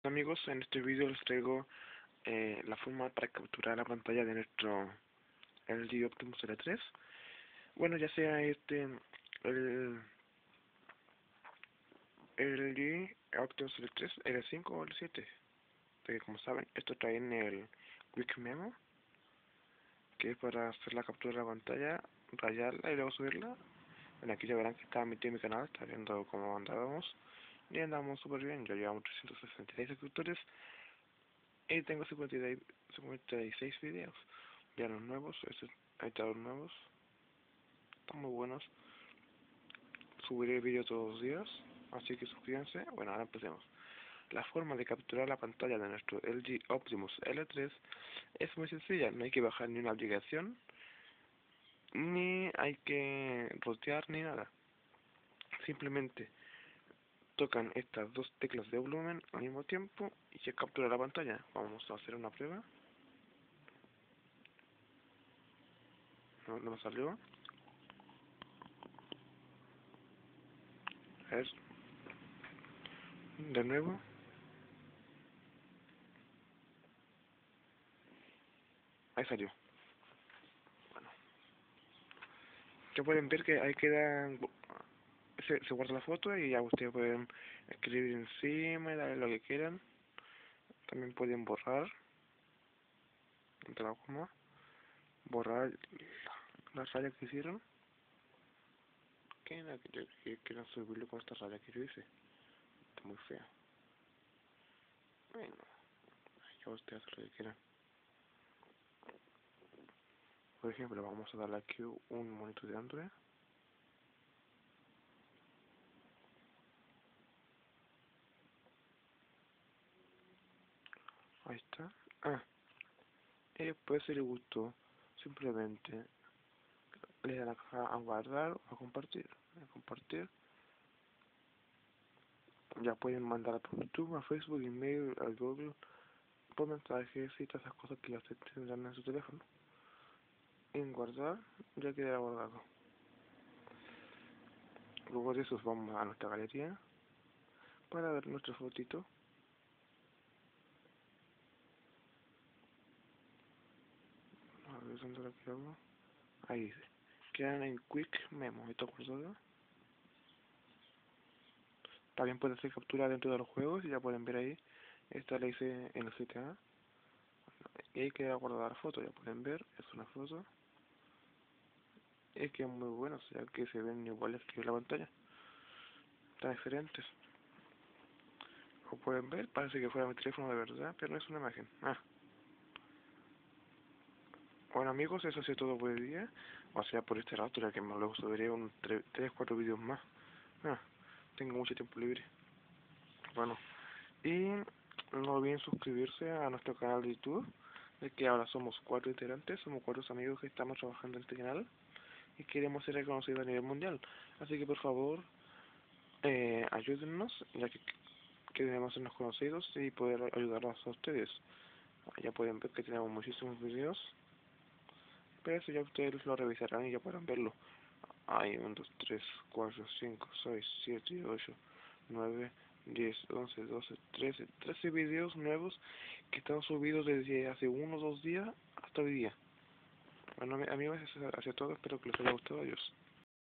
Capturar pantalla de lg l3
Debdn apretar al mismo tiempo los botones del volumen muy rapidamente